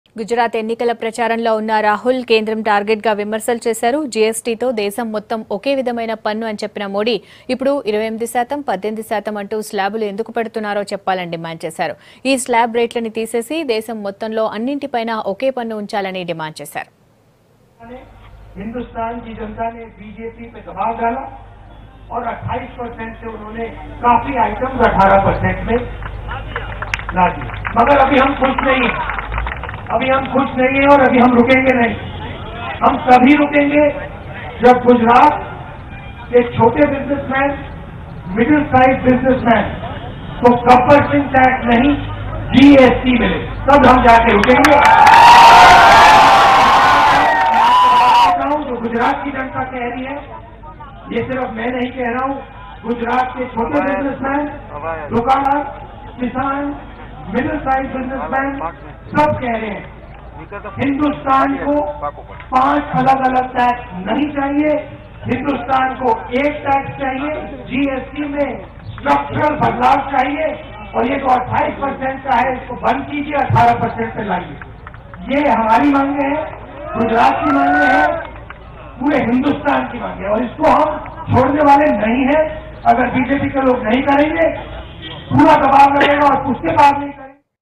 In 2019, We Will Reduce GST Rate To Just One Slab of 18%, says Rahul Gandhi In Gujarat | V6 News
In 2019, We Will Reduce GST Rate To Just One Slab of 18%, says Rahul Gandhi In Gujarat.